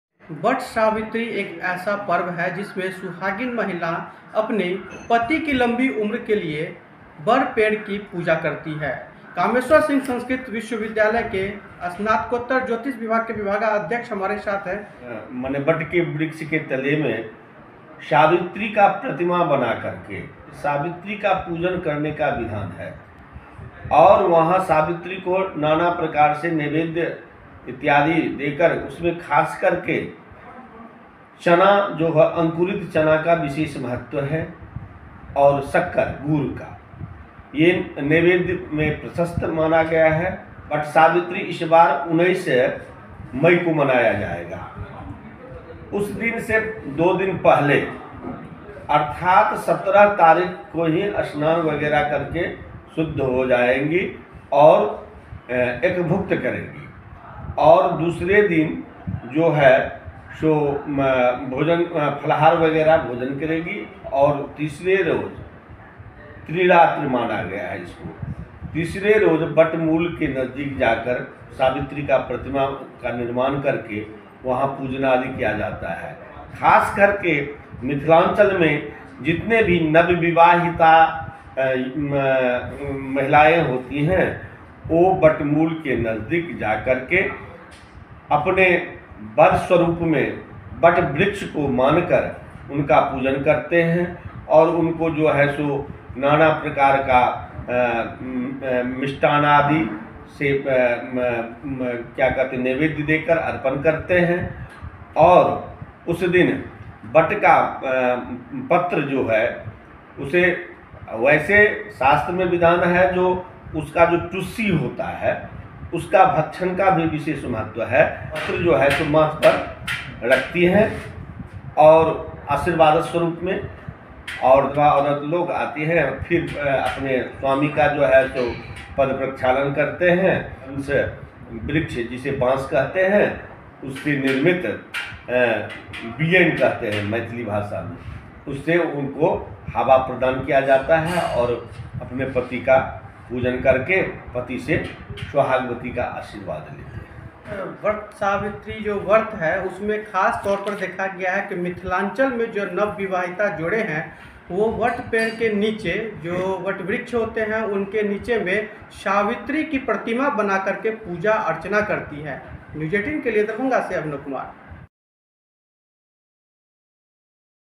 Vat Savitri Vrat 2023: इस विधि से करें वट सावित्री व्रत, होगी मनोकामना पूरी, बढ़ेगी पति की आयु
मिथिलांचल में नवविवाहिता वट मूल के नजदीक जाकर अपने वर स्वरूप में वट वृक्ष को मानकर उनका पूजन करती हैं और मिठाई अर्पण करती हैं. उस दिन वट पत्र जिसे तुसी कहा जाता है. उसे खाने का विशेष महत्व होता है वैसे महिलाएं आशीर्वाद स्वरुप अपने माथे पर वट वृक्ष के पत्ते भी रखती हैं.